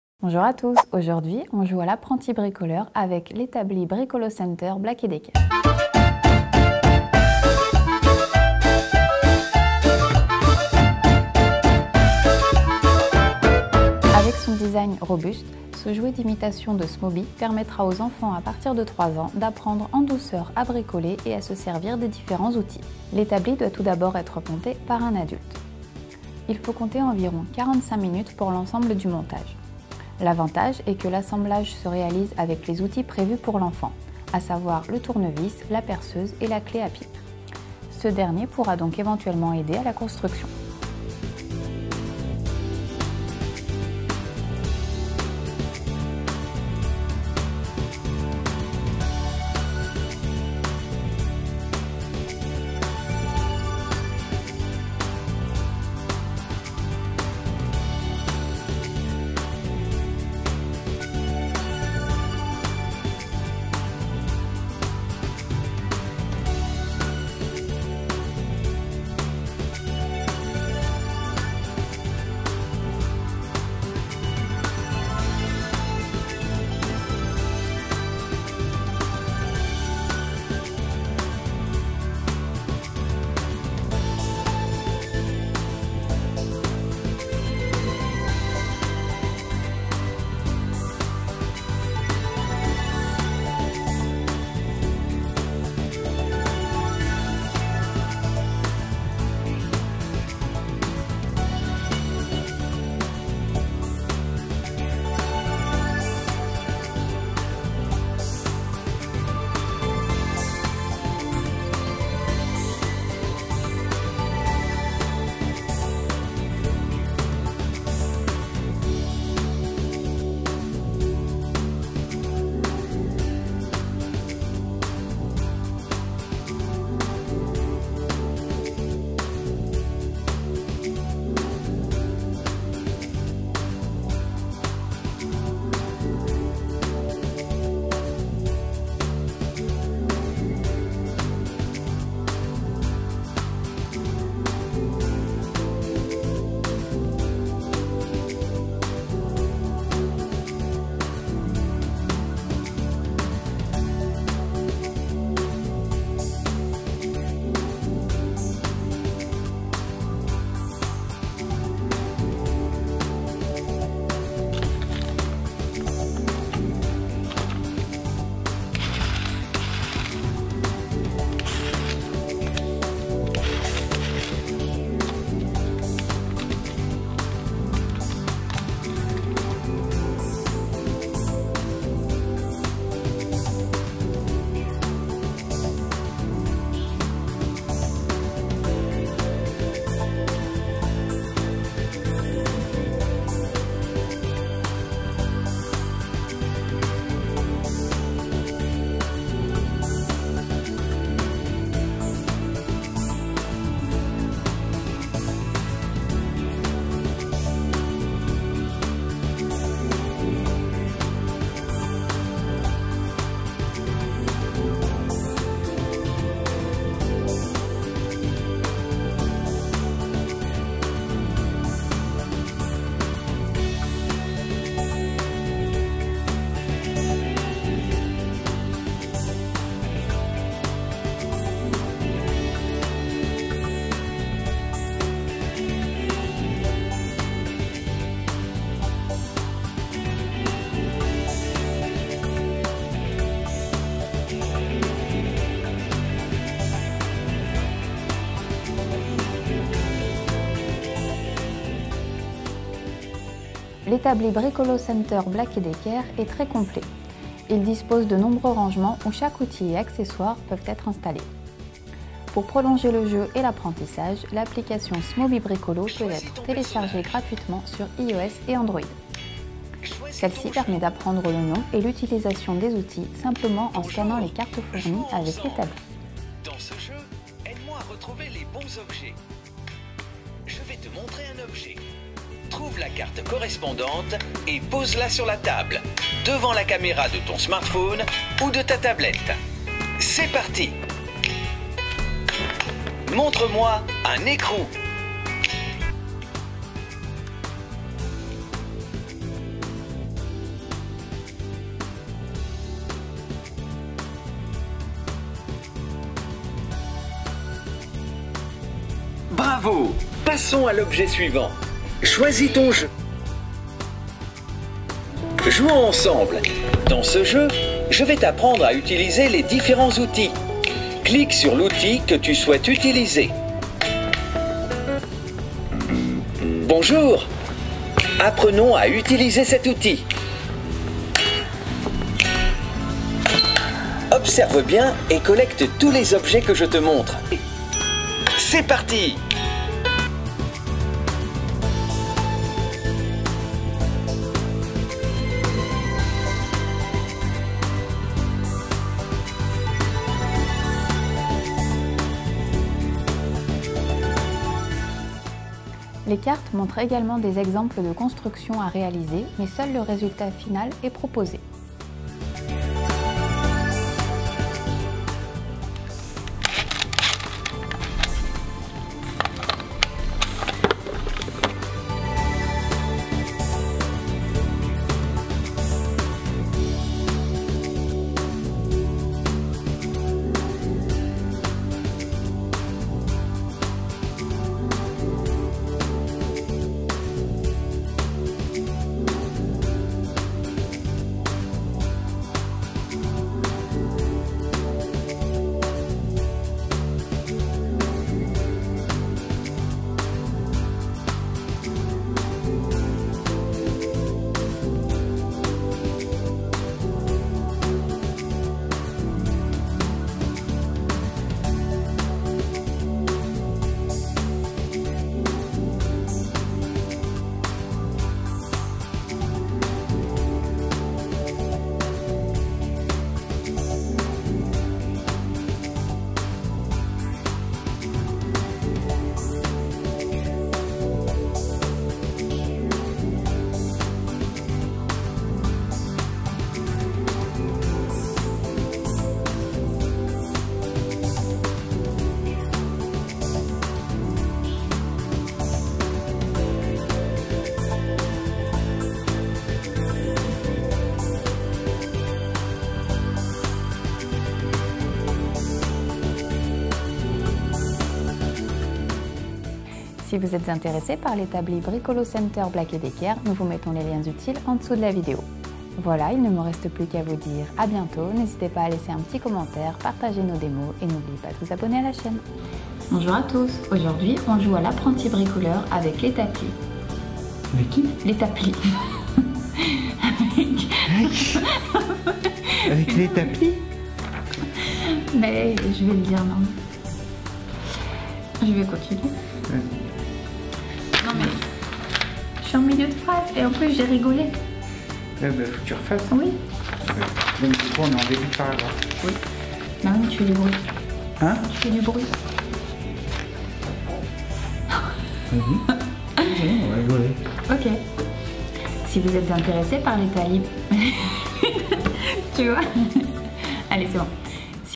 Smoby Bricolo Center Black + Decker - Démo du jouet / établi
Avec son design robuste, ce jouet d’imitation de Smoby permettra aux enfants à partir de 3 ans d’apprendre en douceur à bricoler et à se servir des différents outils. L’établi doit tout d’abord être monté par un adulte. Il faut compter environ 45 minutes pour l’ensemble du montage. L’avantage est que l’assemblage se réalise avec les outils prévus pour l’enfant, à savoir le tournevis, la perceuse et la clé à pipe. Ce dernier pourra donc éventuellement aider à la construction.
L’établi Bricolo Center Black & Decker est très complet, il dispose de nombreux rangements où chaque outil et accessoire peuvent être installés.

Démonstration : Sébastien
Script et Présentation : Marie
Vidéo et montage : Jean-Christophe